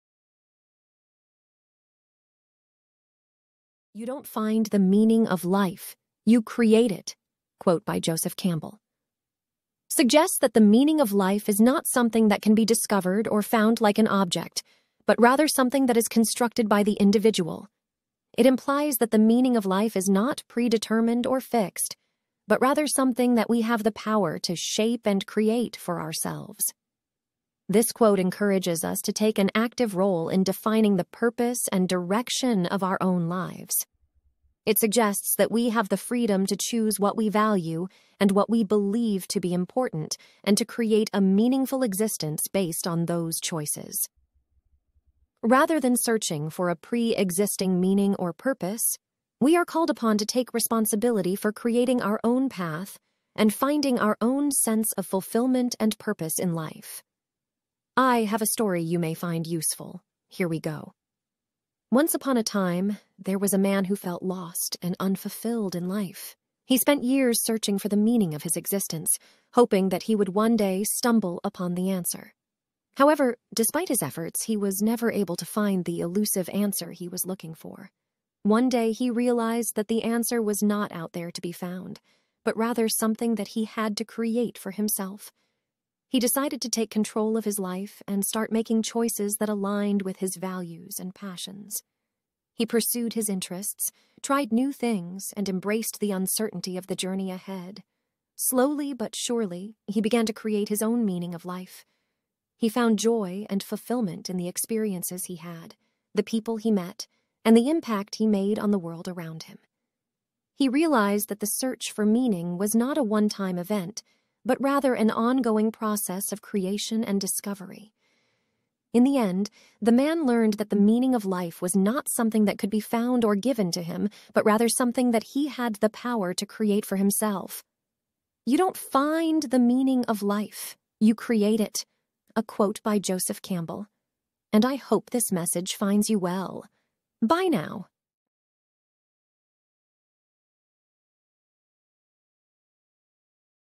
📨 Be the light ☀️
Welcome to the "Are Positive" podcast playlist! In this video, we'll be exploring the deeper meaning behind a powerful quote, and sharing a short story that brings it to life.

So sit back, relax, and let these words of wisdom inspire and uplift you.

Thank you for tuning in, and remember to always stay positive and keep creating your own meaning in life! 🌎